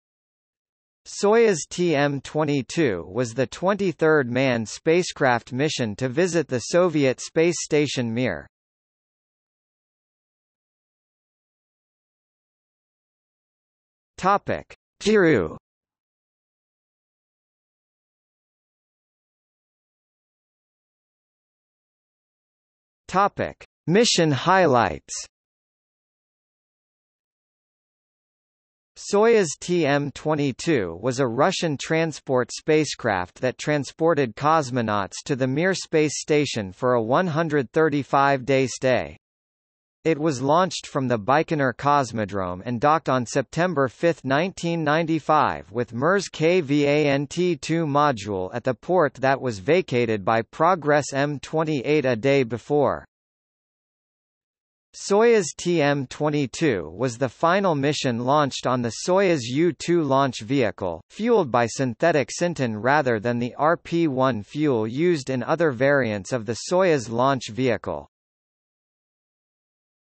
Soyuz TM-22 | Wikipedia audio article
00:00:10 Crew
00:00:19 Mission highlights

- Socrates

SUMMARY
Soyuz TM-22 was the 23rd manned spacecraft mission to visit the Soviet Space Station Mir.